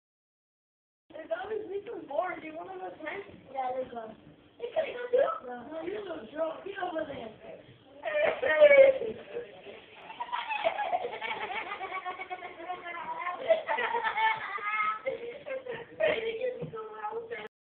Little kid crying can't play
Hewas crying because Gabi and Edgar didn't let Eduardo play